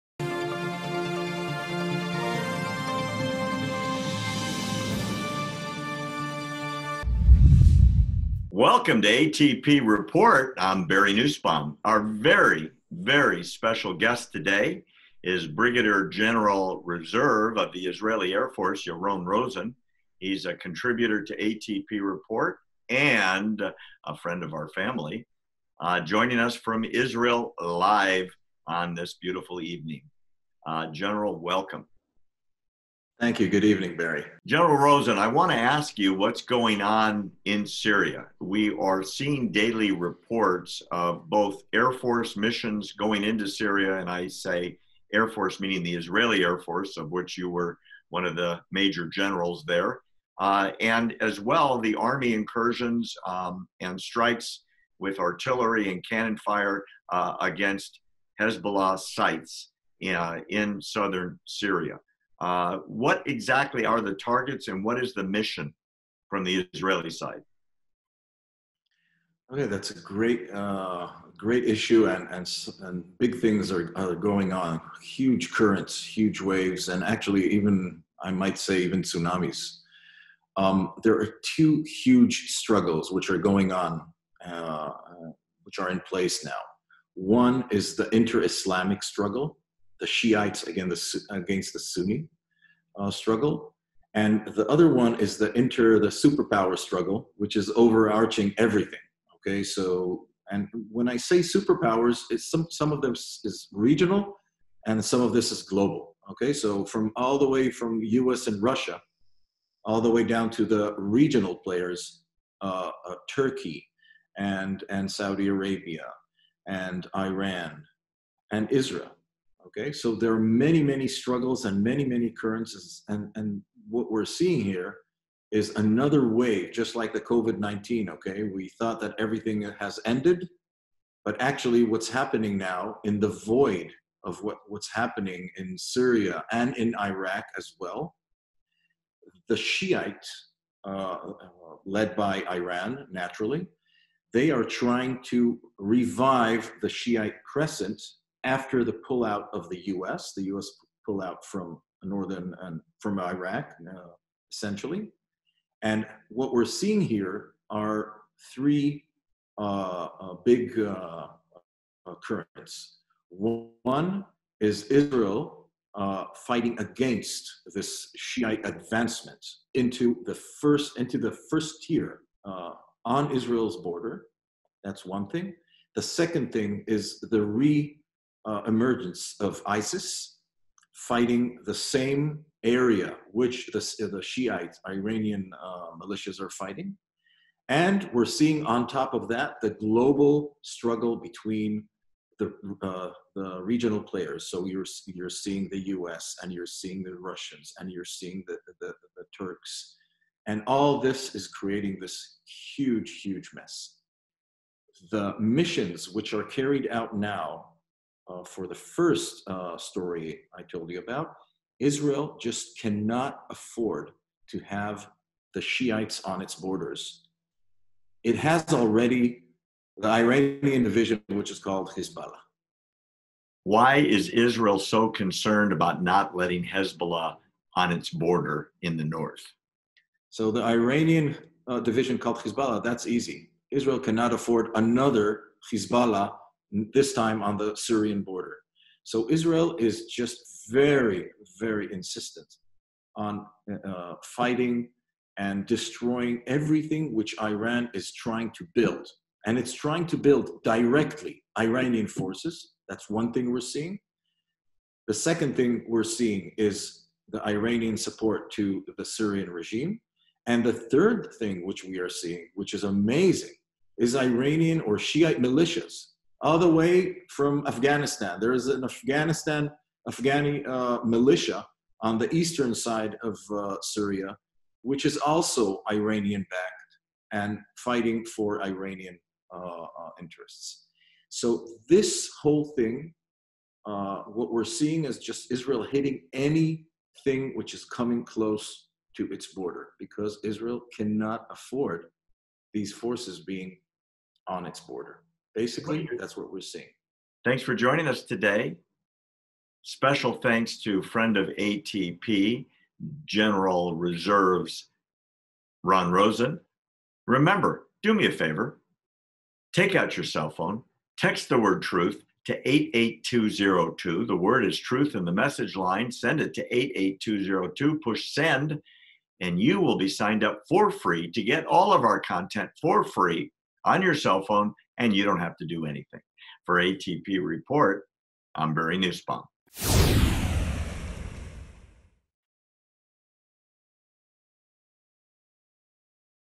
Iranians Attacked by Israel in Syria Explained by IDF General
An ATP Report Production - In this episode of the APT Report, Barry Nussbaum is interviewing IDF General (Res) Yaron Rosen live from Israel. Barry asks the General to explain what is going on in Syria.